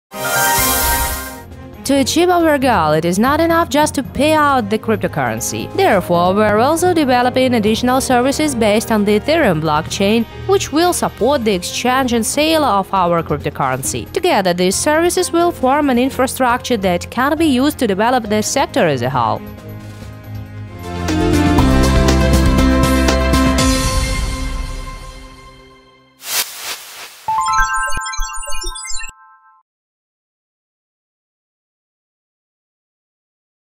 Quote from PlusCoin Whitepaper
To achieve our goal, it is not enough just to pay out the cryptocurrency, therefore we are also developing additional services based on the Ethereum blockchain, which will support the exchange and sale of our cryptocurrency. Together, these services will form an infrastructure that can be used to develop the sector as a whole.